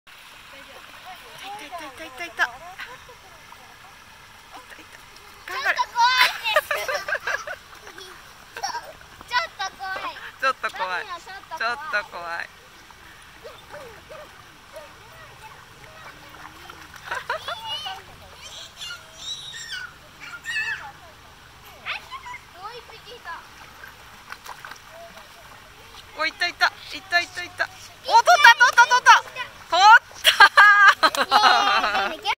保護犬ドミノドッグプール短編集！ すこーしだけ慣れてきました！！🐕🌊
少し慣れてきて、投げた輪っかも取れました！！！！